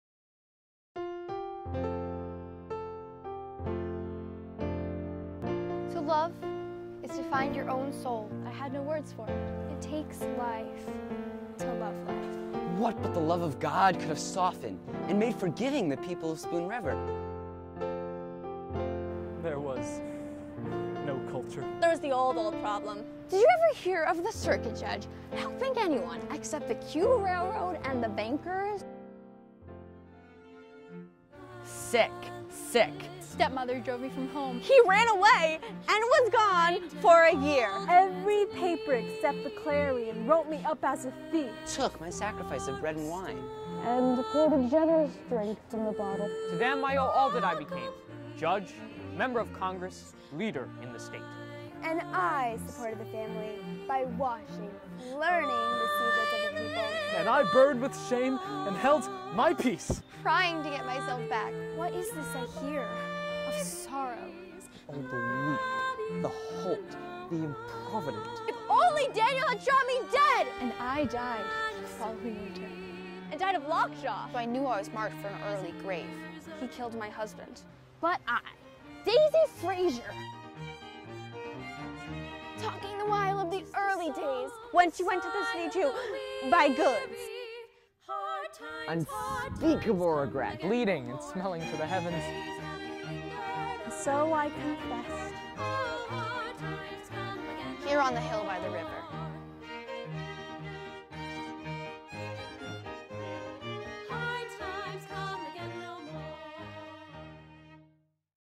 The Spoon River Project (Trailer)
Enjoy our trailer for our fall play, The Spoon River Project!